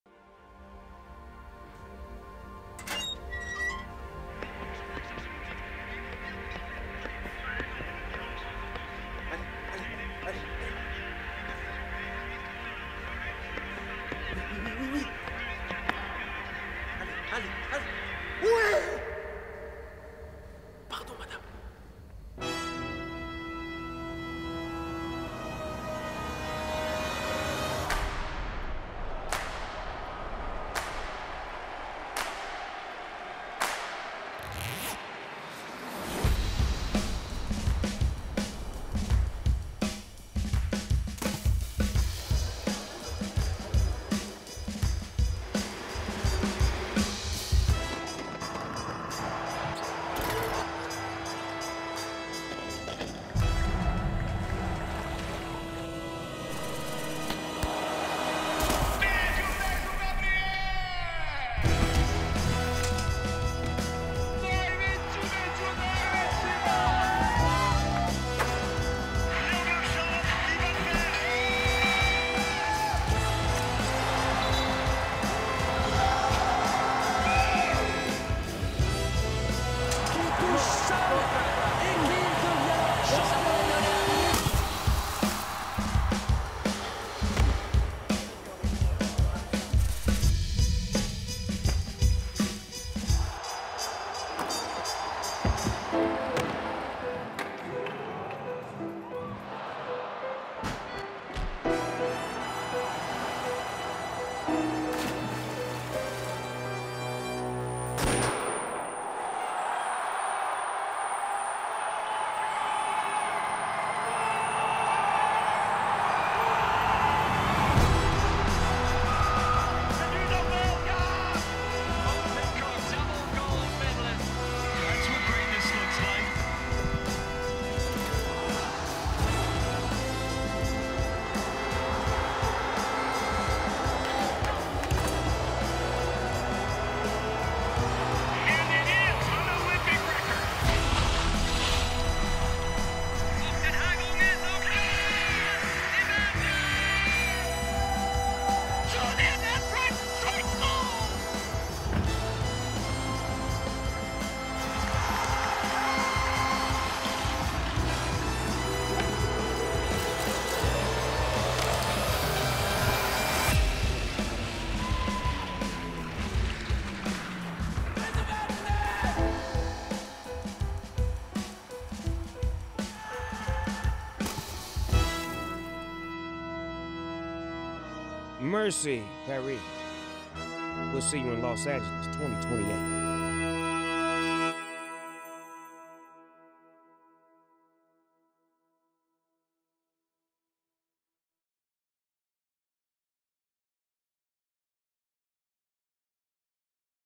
Memories of Paris2024 💙
Experience the most unforgettable highlights and moments from the Paris 2024 Olympic Games!